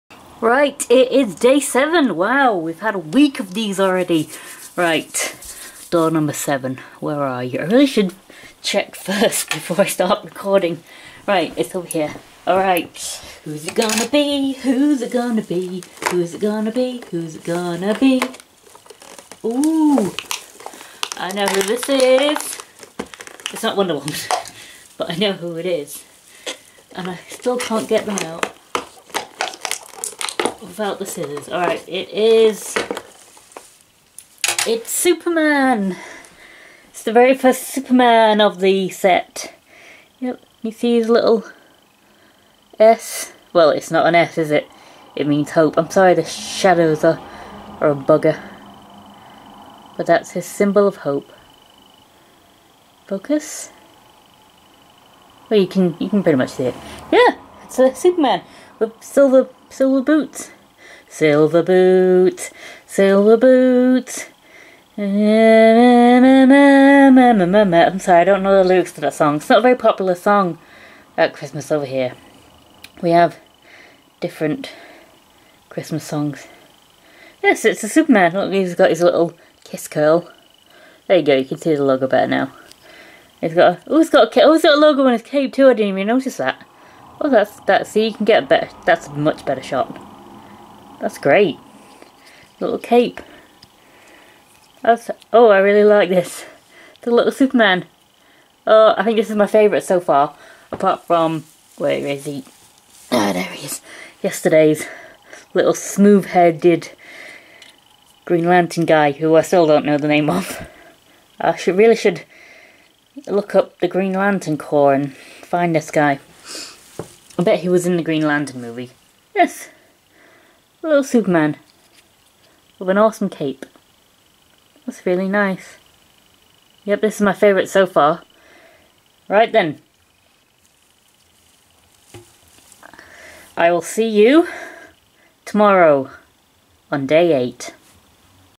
DC Ooshies advent calendar 2018: Day 7
It's a bird! It's a plane! No It's a Superman Ooshie!!